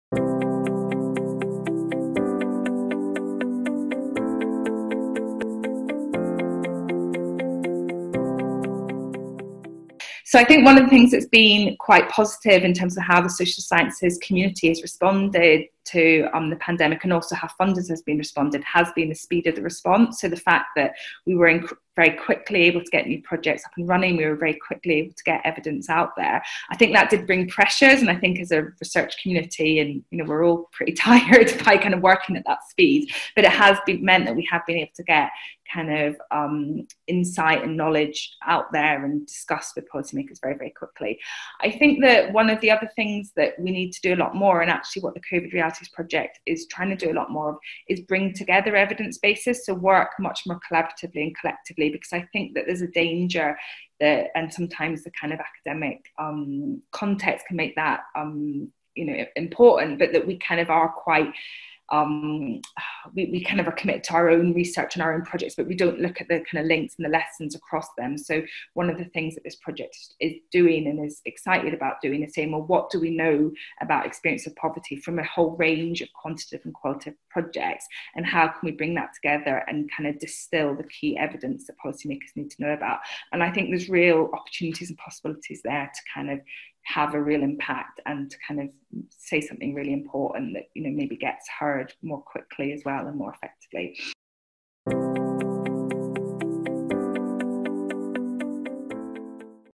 Ruth Patrick on the importance of the social sciences during the Covid-19 pandemic
Ruth Patrick, Lecturer in Social Policy and Social Work at the University of York, answers questions about her research into how low-income families are navigating the Covid-19 crisis.